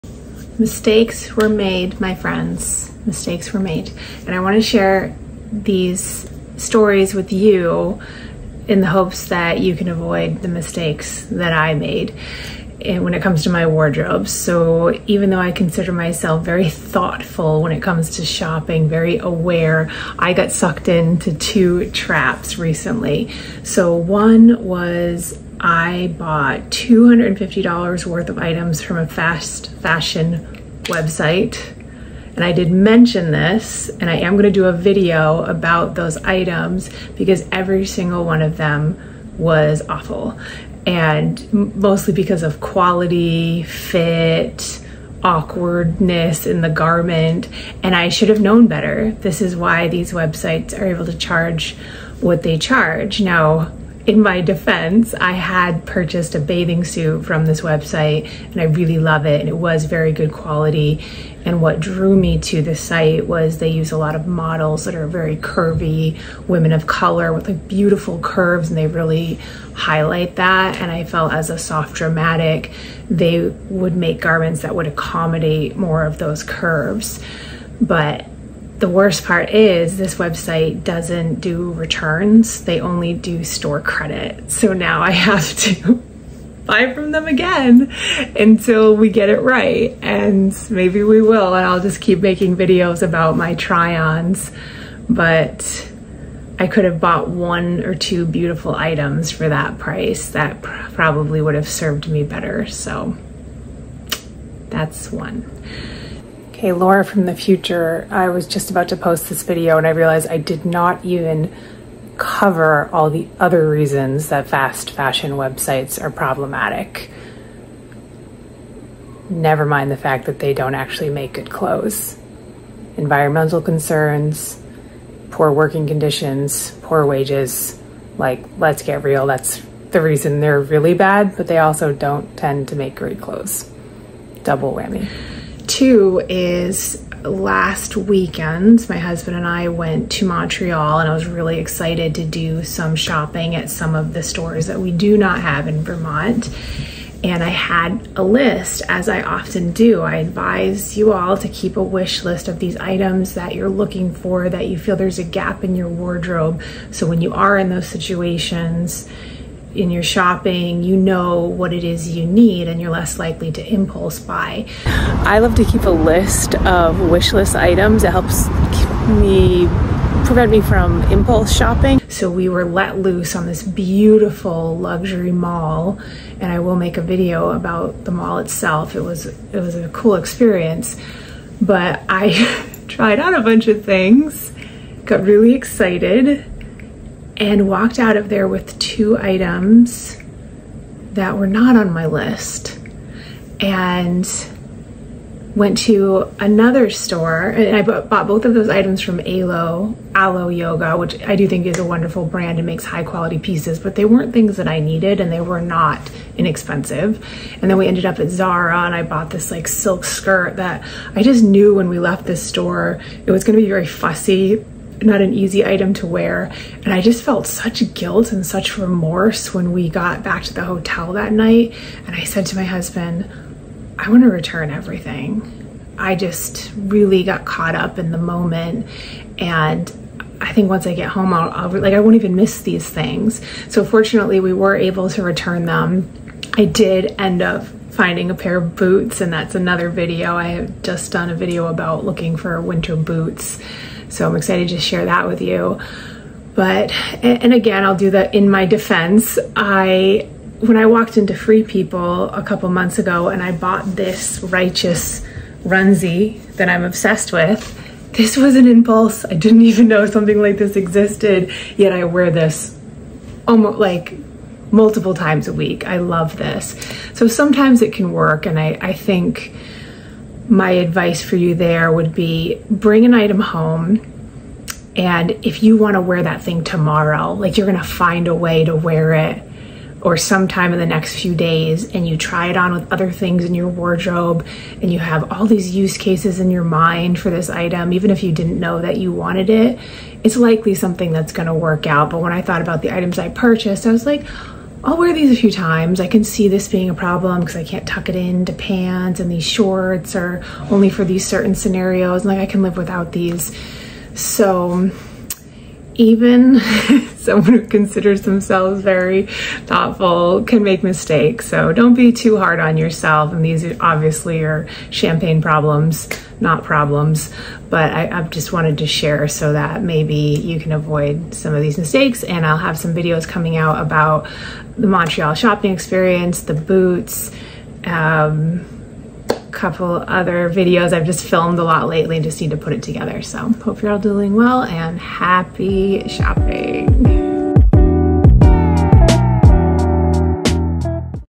Mistakes Were Made - Don’t Do What I Did!
Even someone who puts a lot of thought into styling and building my wardrobe mindfully gets caught in these classic fashion traps. Watch this video and don’t do what I did!